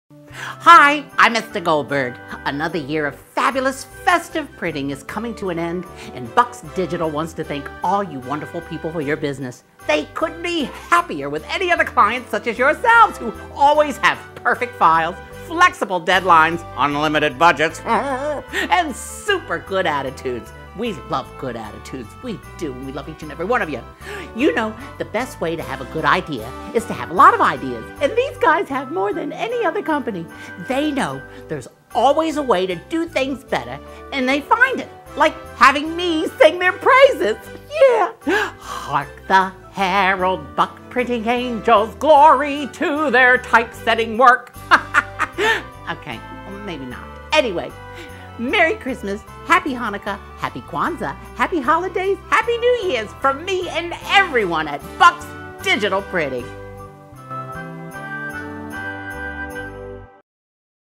Bucks Digital Printing Holiday 2012 with Ester Goldberg
Ester Goldberg wishes all of our customers a happy holiday